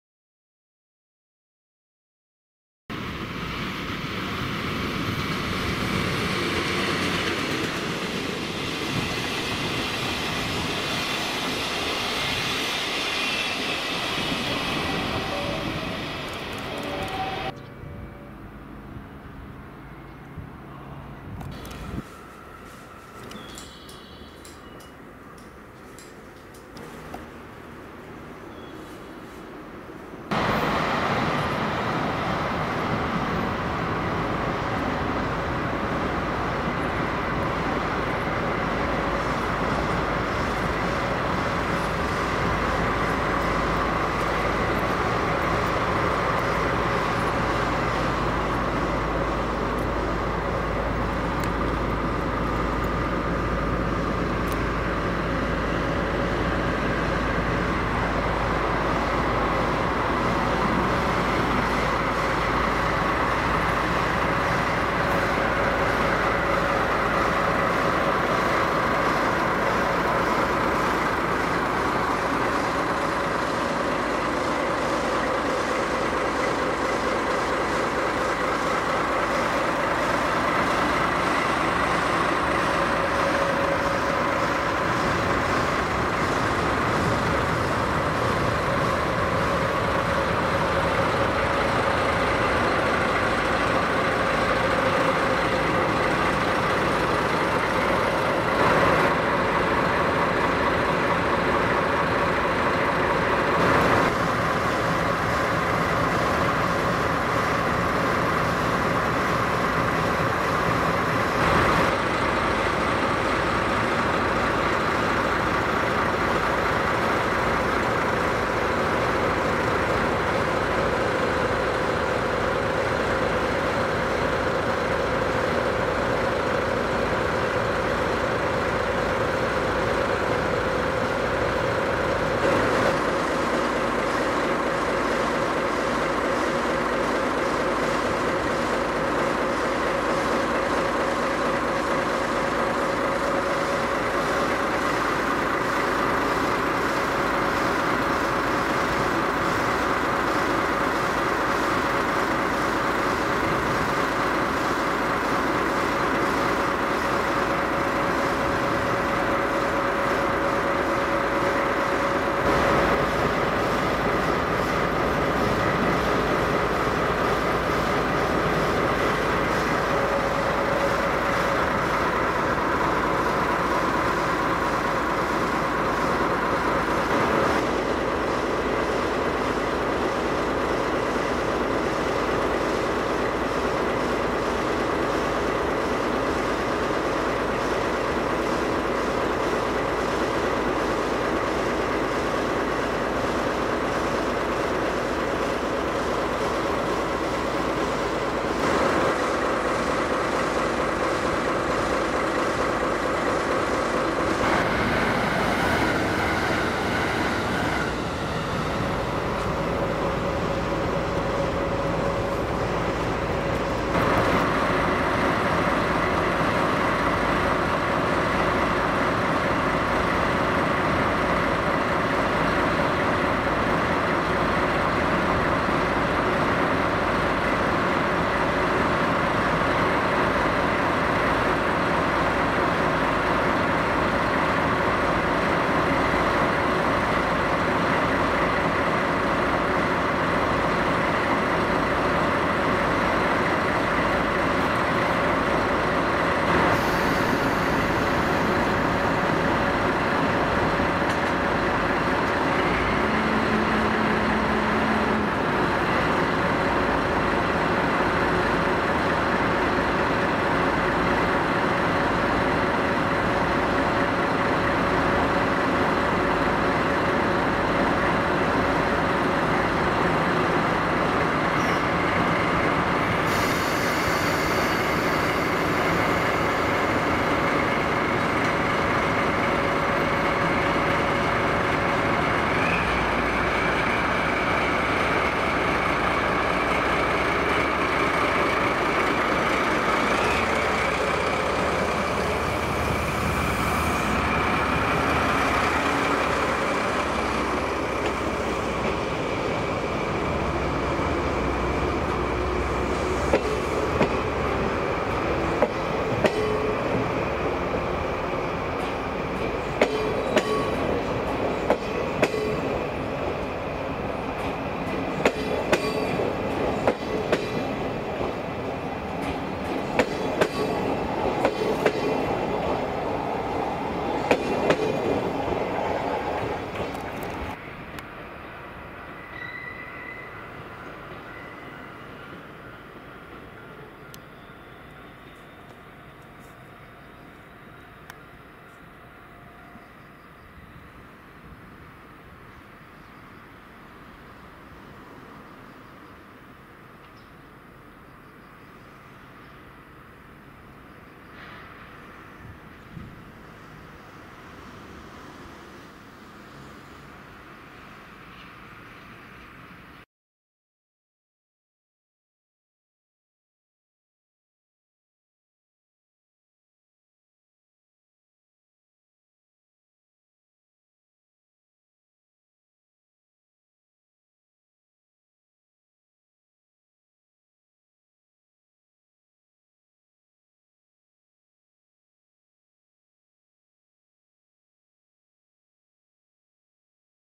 【4K/網干訓練】乗務員訓練(往路)/Train driver training(outward) 2020.11.11
On November 11, 2020, I took a video of DD51-1193 and 12 series passenger cars×5 at Takatori Station and Gochaku Station.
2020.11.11 DD51-1193号機と12系客室5両を鷹取駅と御着駅にて収録させていただきました。
ご視聴いただきありがとうございますm(_ _)m